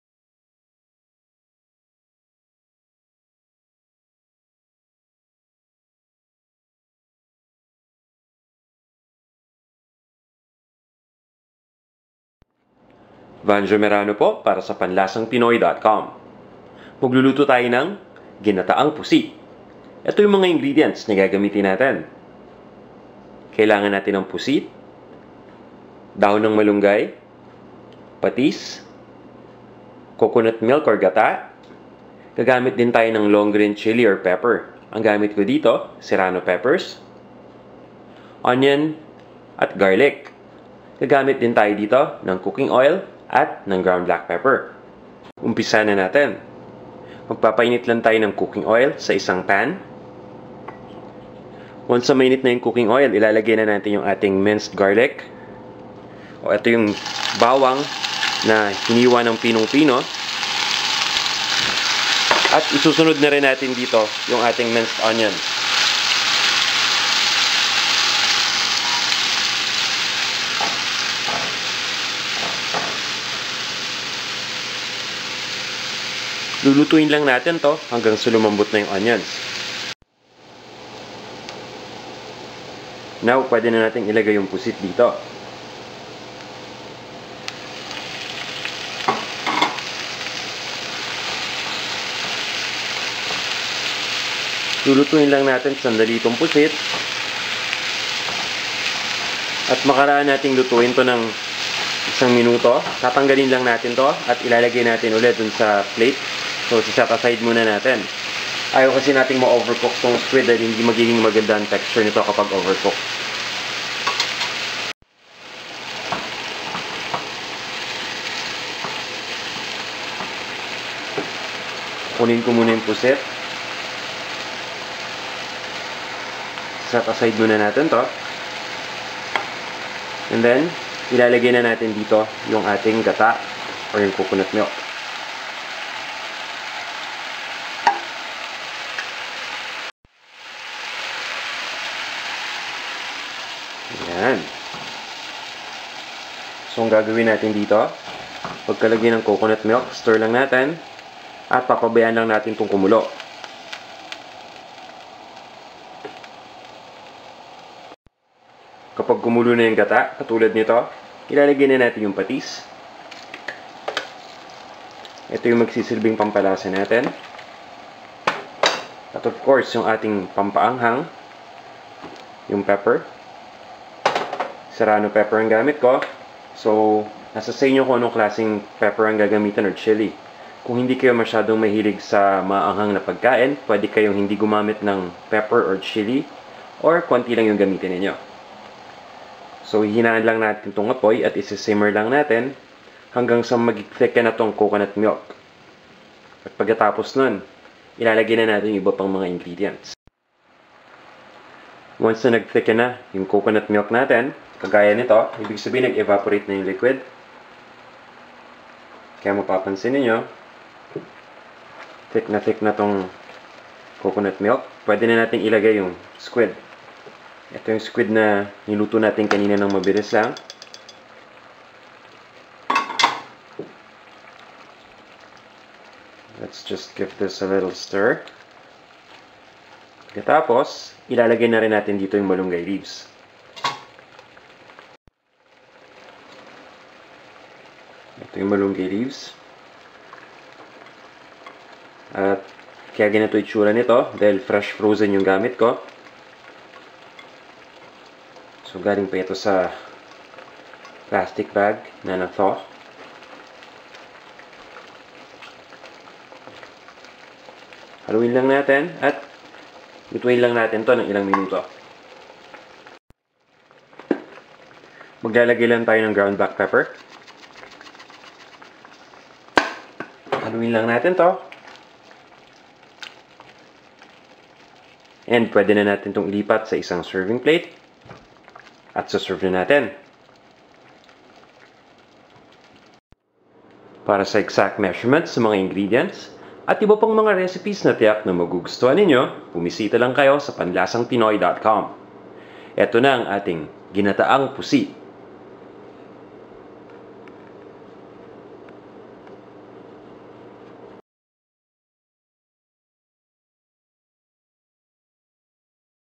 Ginataang Pusit
Ginataang Pusit is Squid cooked in coconut milk. This is a delicious Filipino recipe that you should try.